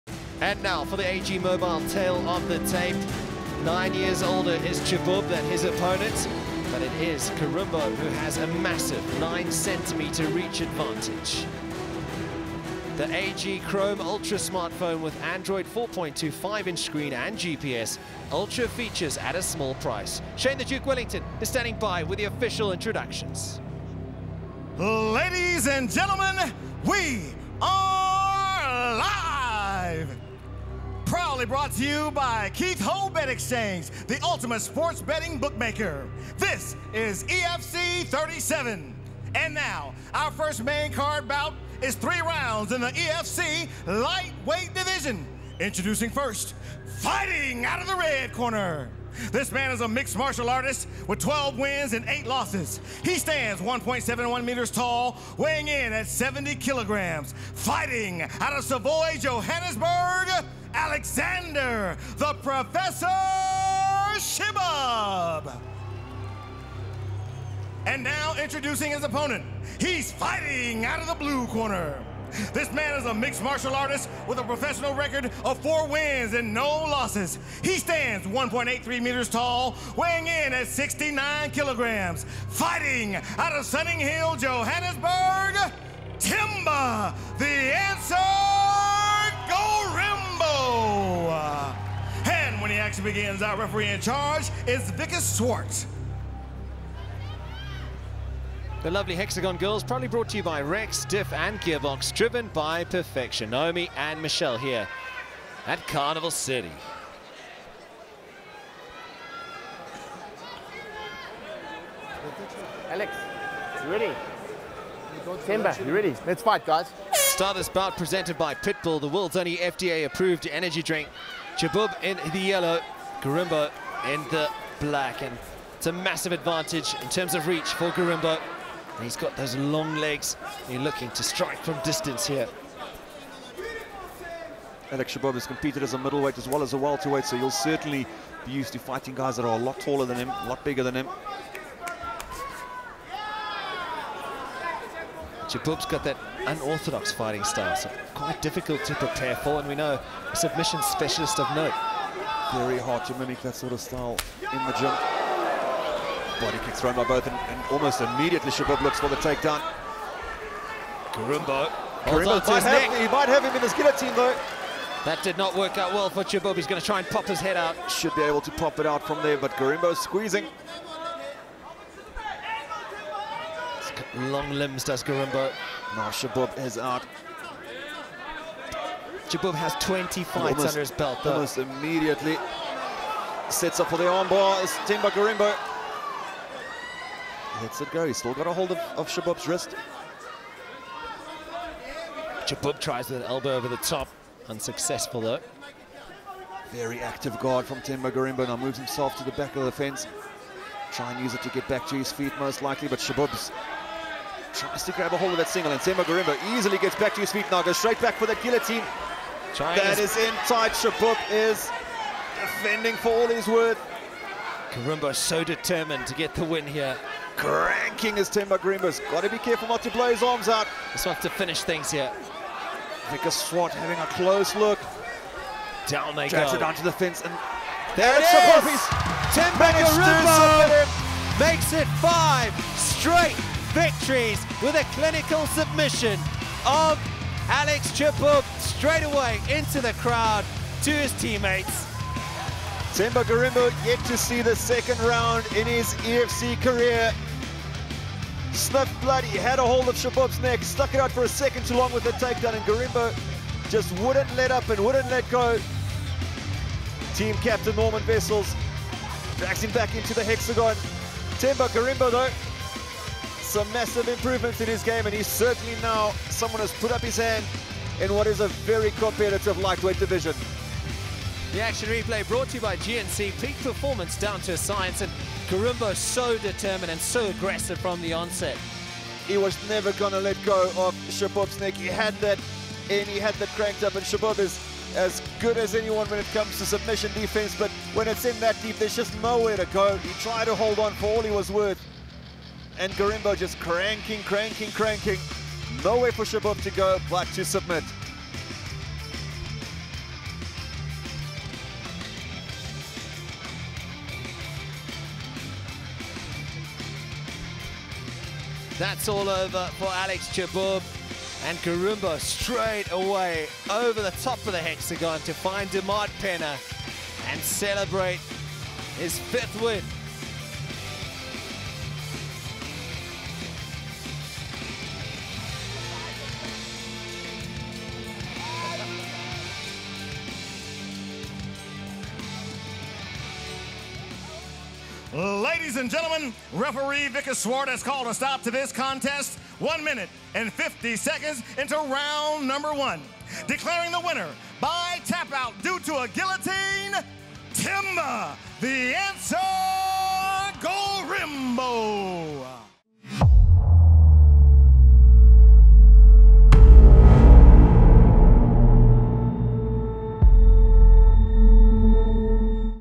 EFC 79 Free Fight Gorimbo vs Cheboub
On Saturday 04 May, Zimbabwe's Themba Gorimbo faces Brazil's Jose Da Rocha in the EFC welterweight Grand Prix for a guaranteed shot at EFC gold. Relive his submission victory over Alex Cheboub.